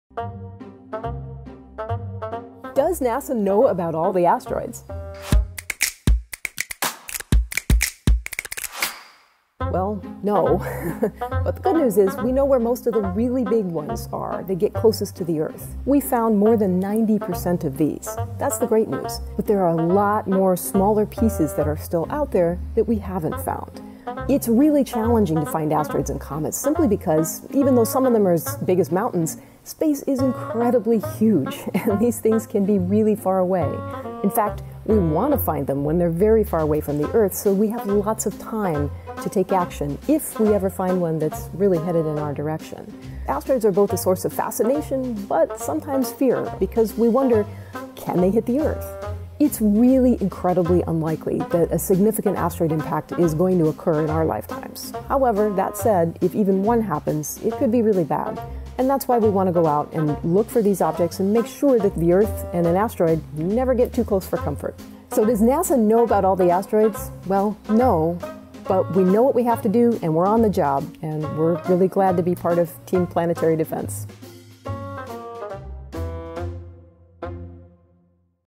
We Asked a NASA Scientist – Does NASA Know About All the Asteroids
Does NASA know about ALL the asteroids? We know about the vast majority of larger ones and none of those pose a threat, but space is big, so we're always on the lookout. NASA asteroid expert Dr. Amy Mainzer explains.

Credit: NASA

Producer: Scott Bednar
Producer/Editor: Jessica Wilde